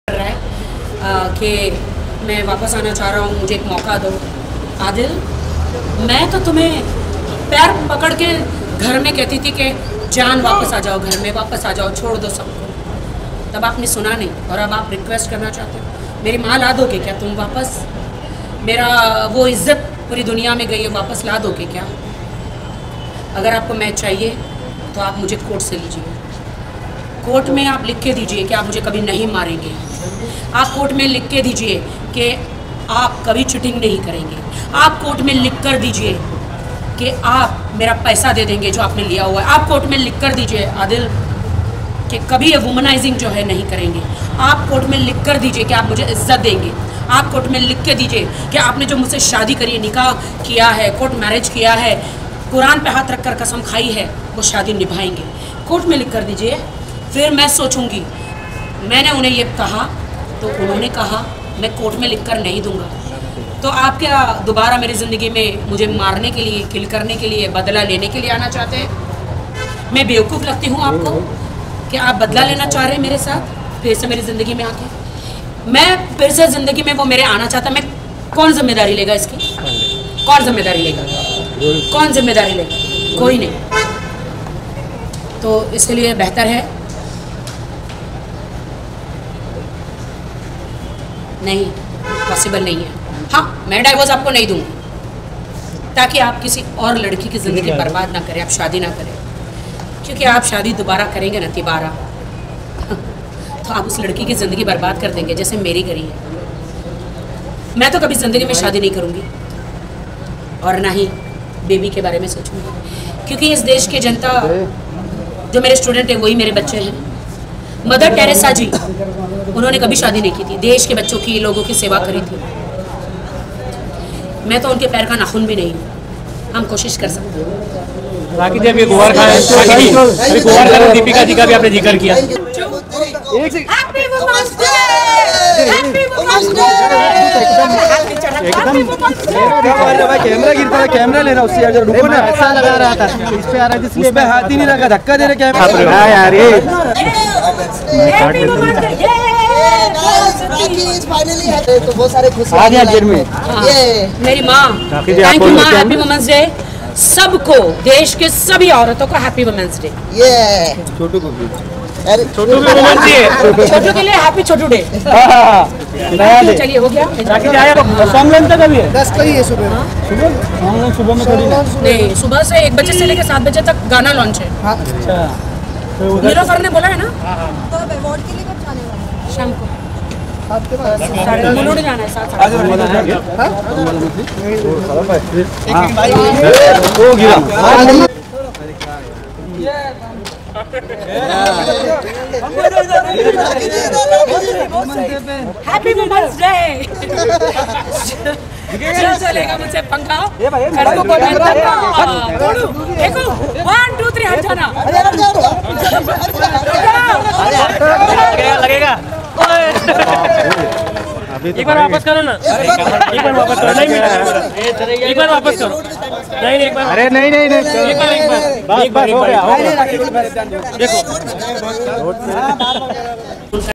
आदिल मुझे कोर्ट से लीजिये - राखी ने फिर पति आदिल पर ये क्या कह दिया Rakhi Sawant On Adil Khan
मुझे कोर्ट से लीजिये - राखी ने फिर पति आदिल पर ये क्या कह दिया Rakhi Sawant On Adil Khan